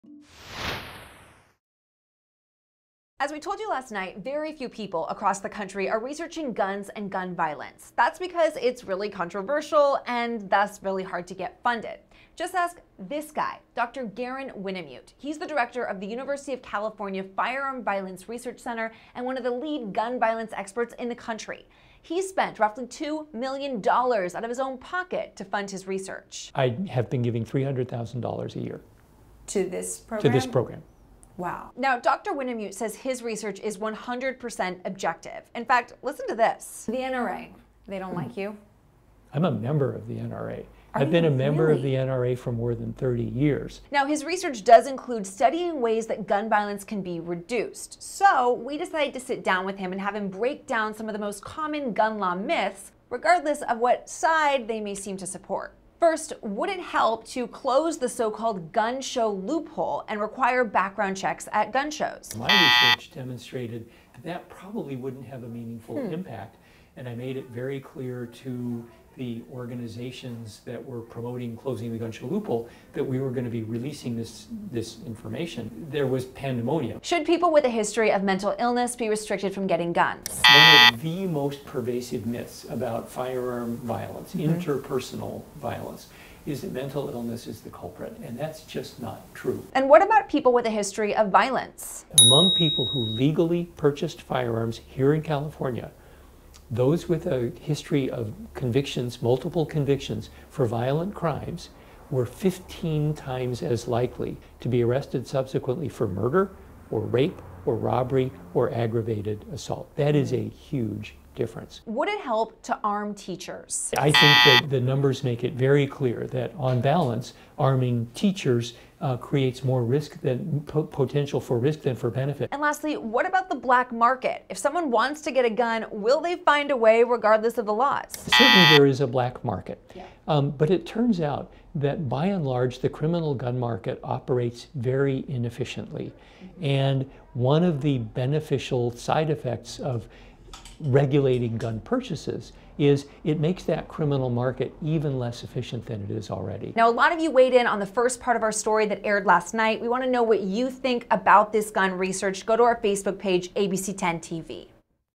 Local expert on firearms breaks down gun law myths
Local expert on firearms breaks down gun law myths (March 15, 2018)